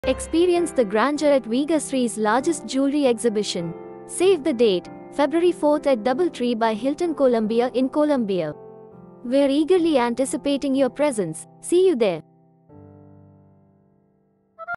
𝐕𝐄𝐆𝐀 𝐒𝐑𝐈 𝐆𝐎𝐋𝐃 & 𝐃𝐈𝐀𝐌𝐎𝐍𝐃𝐒 𝐂𝐨𝐦𝐢𝐧𝐠 𝐭𝐨 Columbia... 𝐓𝐡𝐢𝐬 𝐰𝐞𝐞𝐤𝐞𝐧𝐝💎
⚡𝐓𝐡𝐞 𝐥𝐚𝐫𝐠𝐞𝐬𝐭 𝐚𝐧𝐝 𝐭𝐡𝐞 𝐦𝐨𝐬𝐭 𝐞𝐱𝐜𝐥𝐮𝐬𝐢𝐯𝐞 𝐣𝐞𝐰𝐞𝐥𝐥𝐞𝐫𝐲 𝐞𝐱𝐡𝐢𝐛𝐢𝐭𝐢𝐨𝐧 𝐚𝐭 Columbia.

Get ready to be captivated by our exquisite jewelry collection on 4th Feb, 2024.

📌Don’t miss this incredible opportunity to witness the beauty and craftsmanship of our stunning pieces.

Mark your calendars and make your way to :

Venue - Double Tree by Hilton Columbia, 5485 Twin Knolls Road, Columbia MD , 21045.

Join us for an unforgettable experience!